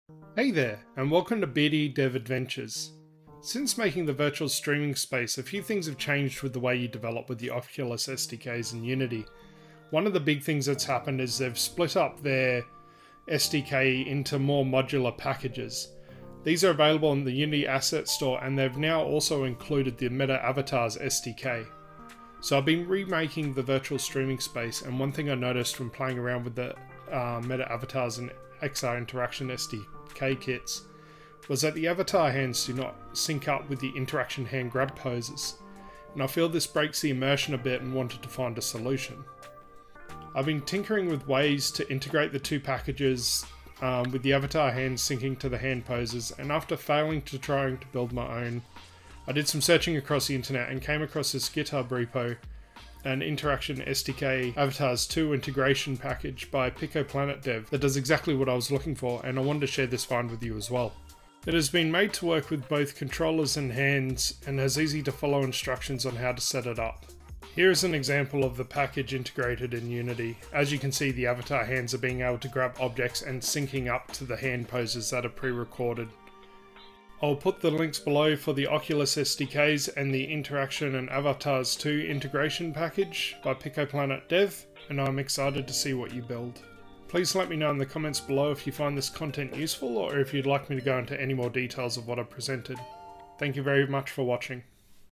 How to Sync Meta Avatars SDK Hands with XR Interaction SDK Poses for Improved VR Experience
In this video, we're showing a GitHub project that will allow you to sync up the hand poses used in the Meta XR Interaction SDK with the Meta Avatars SDK hands to have more realistic holding of items.